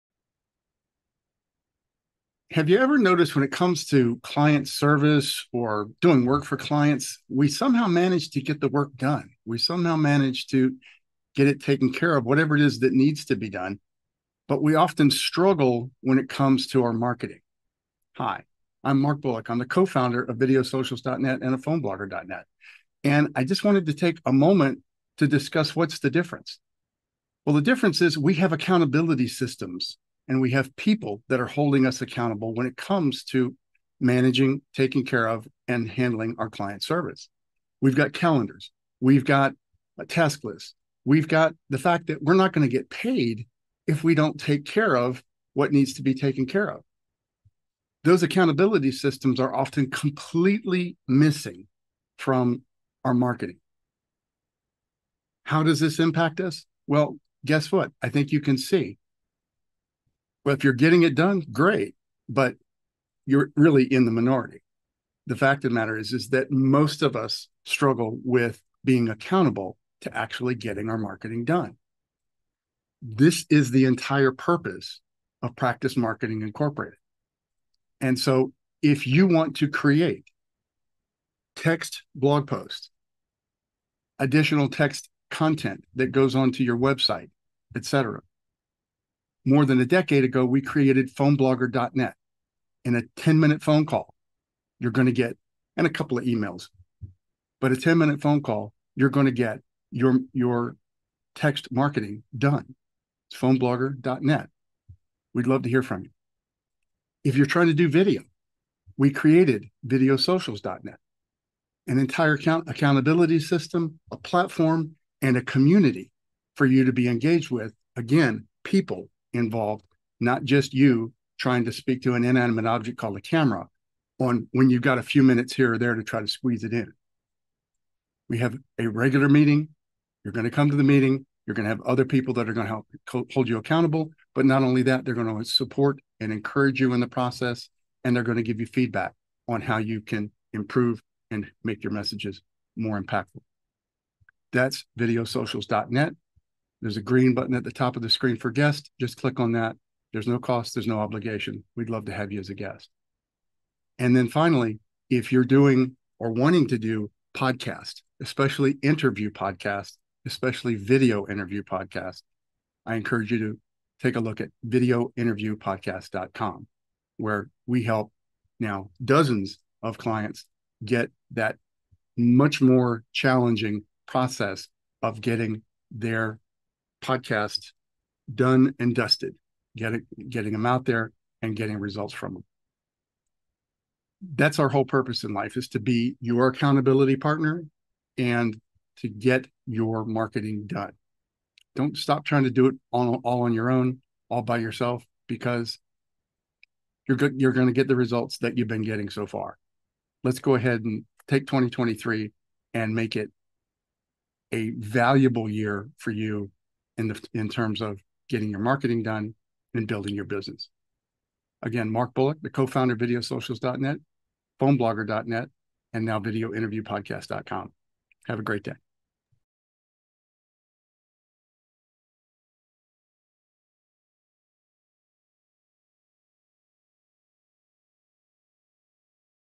Mark Bullock: Systemize Your Marketing and Win
Doing your online marketing on your own? Finding it difficult to get it done, consistently? Mark Bullock, co-founder and COO of Practice Marketing Inc. (PMI) suggests that whatever type of marketing you are engaged in — text, video, or even interview podcasts— PMI has a service to help and support you in maximizing your marketing efforts. PhoneBlogger.net, Videosocials.net, and VideoInterviewPodcast.com provide the support you need to get your marketing done and grow your business.

For help with video blogs: Videosocials.net (Be our guest for free at a club!